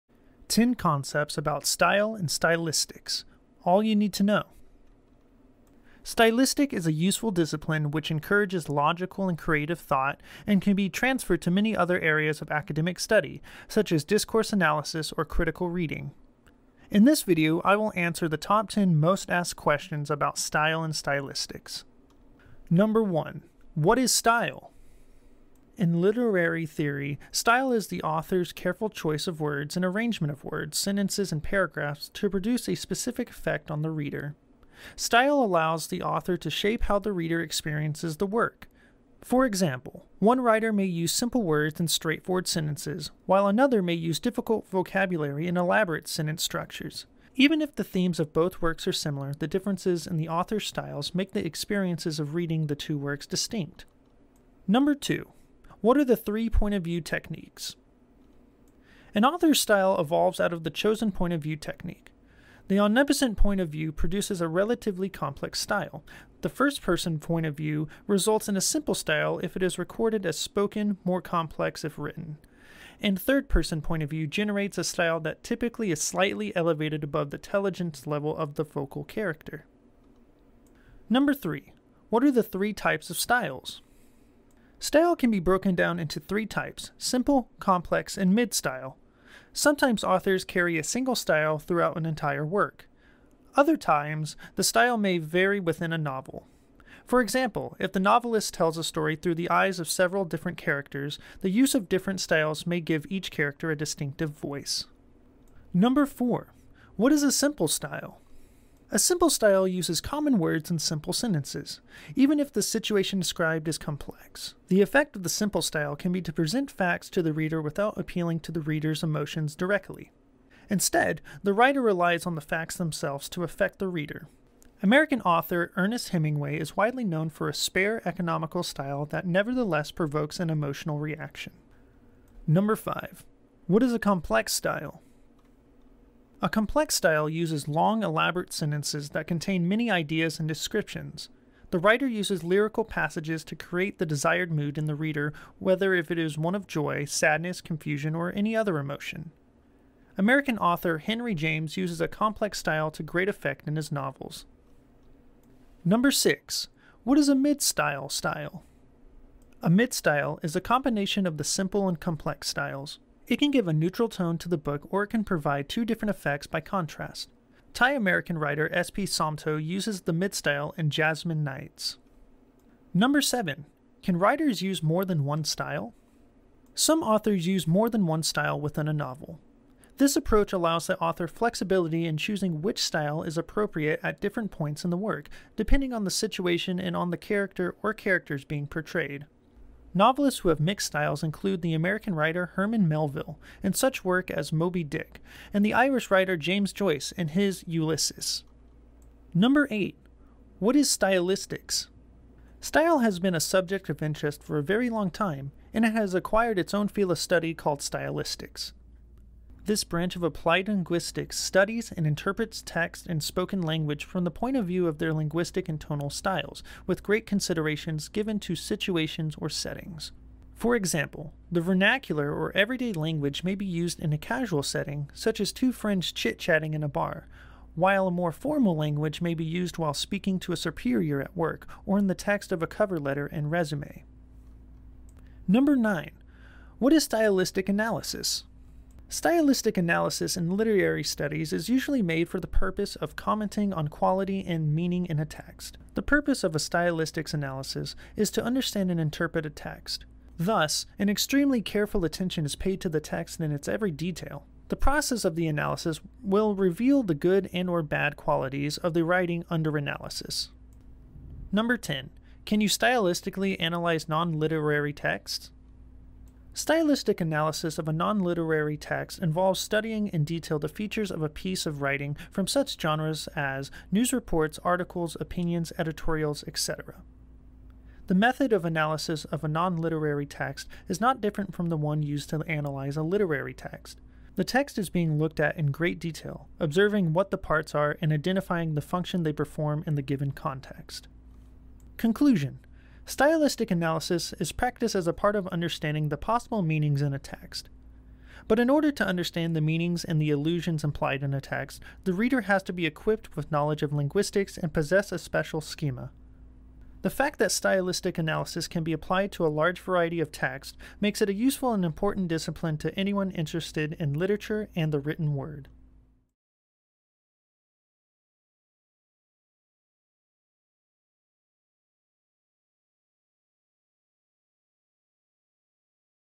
10 Concepts About STYLE and STYLISTICS | All You Need To Know
10 Concepts About STYLE and STYLISTICS | All You Need To Know

In this video I will answer the top 10 most asked questions about style and stylistics.

#1 What is style?

#2 What are the three point-of-view techniques?

#3 What are the three types of styles?

#4 What is a simple style?
#5 What is a complex style?
#6 What is a mid-style style?
#7 Can writers use more than one style?

#8 What is stylistics?

#9 What is stylistic analysis?

#10 Can you stylistically analyze non-literary texts?

Conclusion

I'm a Romanian-born Beijing-based travel vlogger, writer, journalist, and education entrepreneur with an interest in reading literacy, language learning, and world travel.